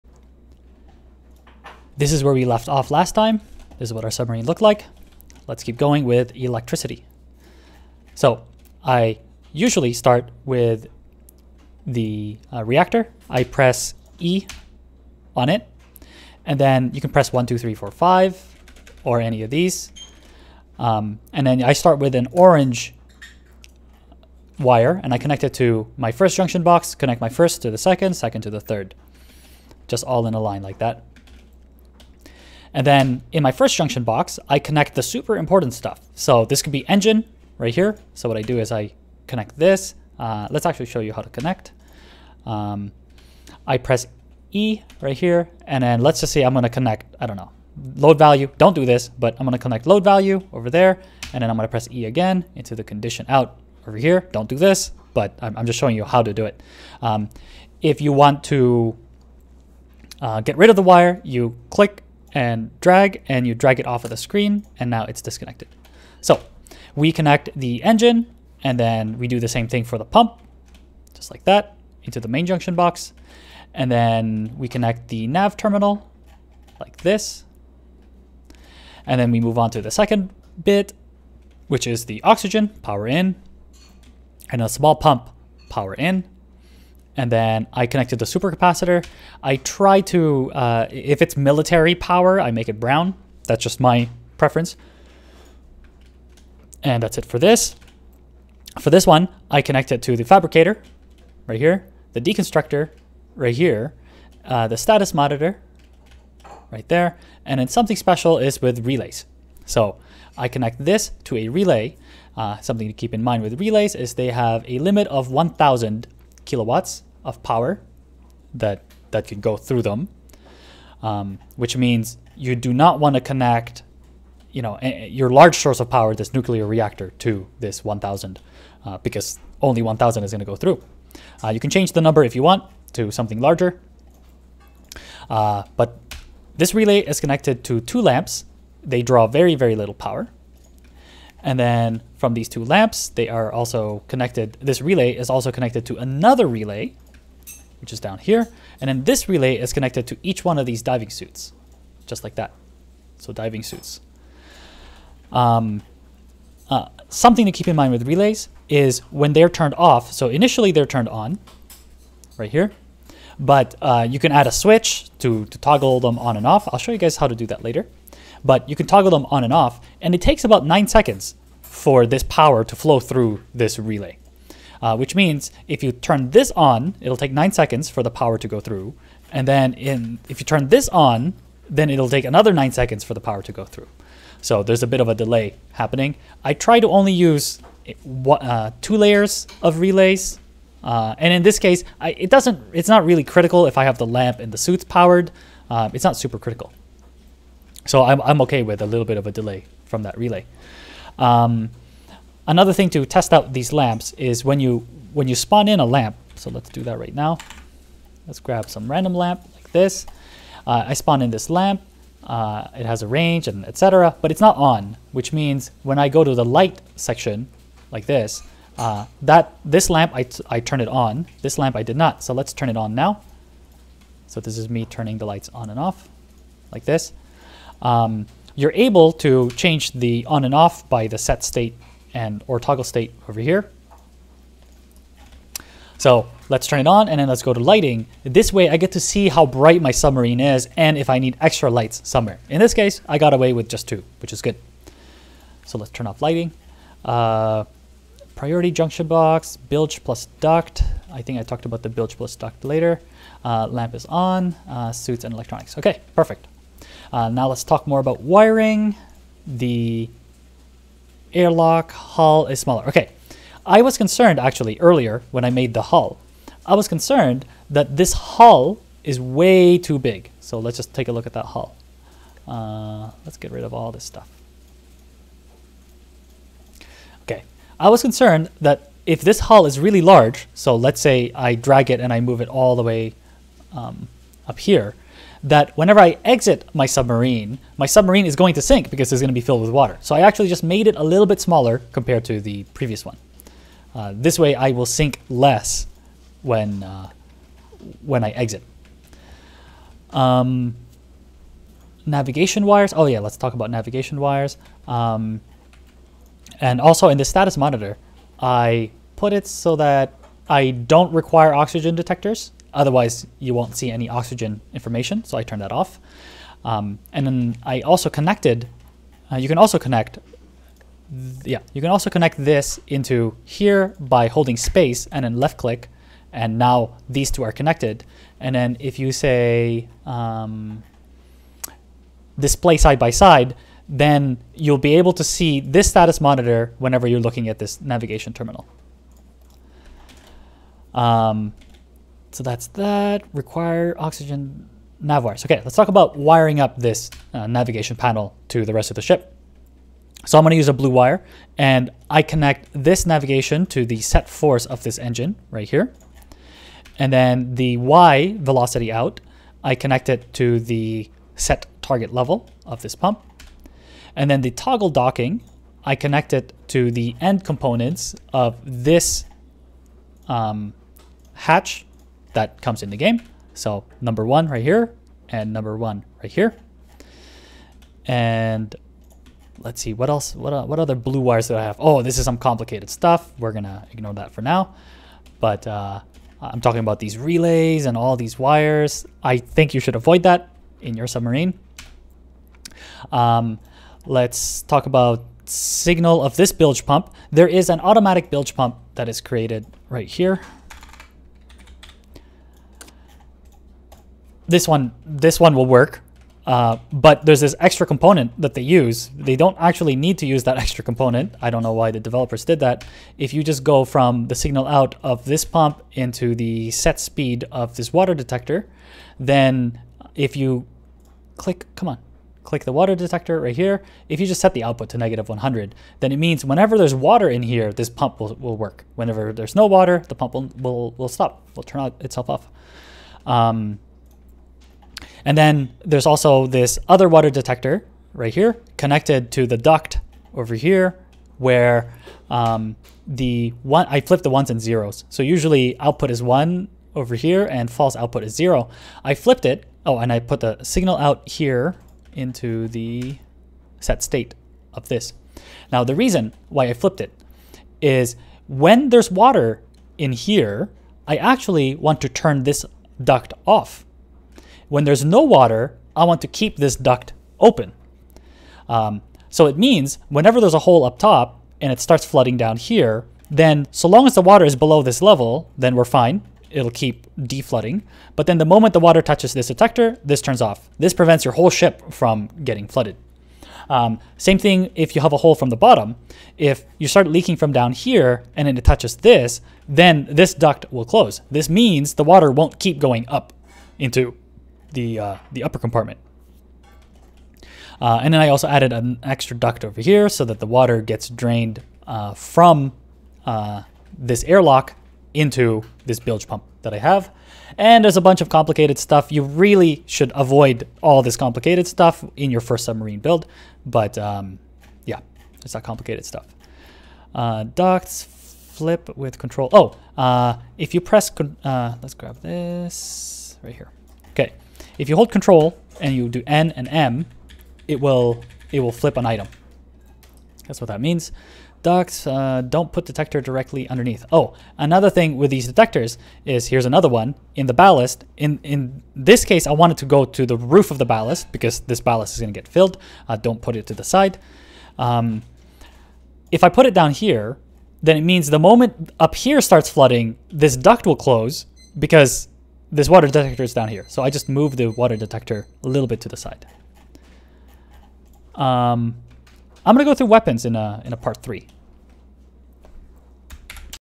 Tiny sub tutorial 2: electricity and wiring
In this guide, I go though
* setting up junction boxes
* connecting power
* connecting navigation controls